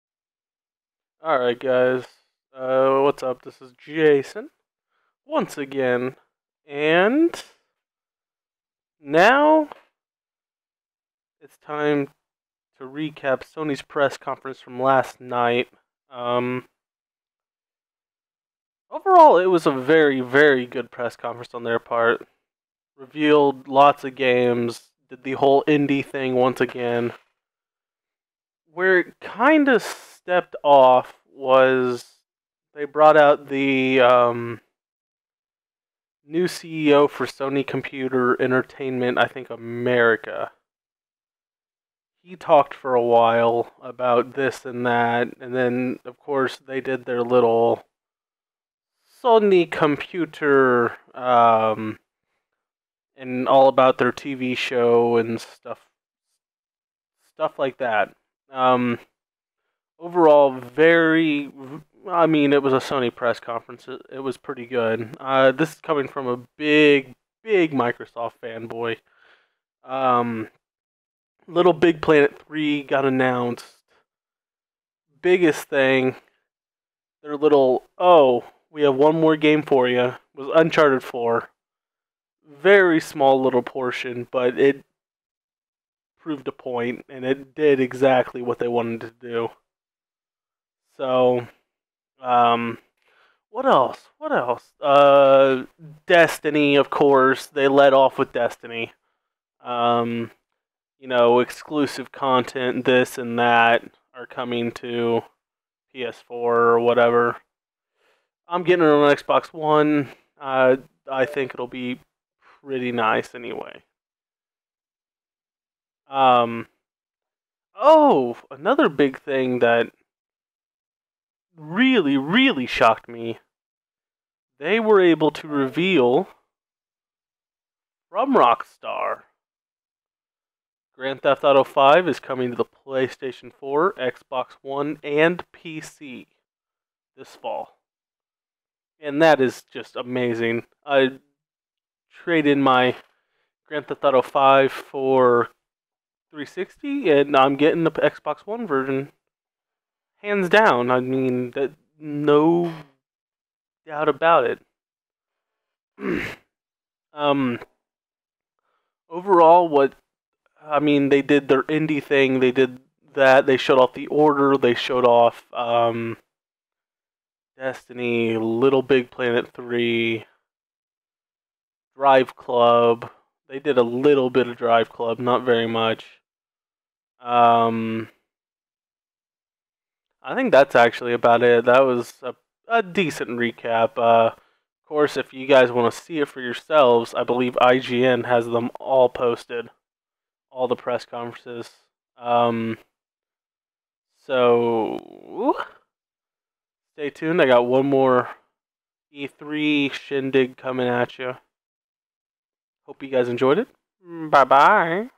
Sony E3 2014 Recap
Here finally is the Sony press conference recap.  I got one more video for you but that might wait till tomorrow.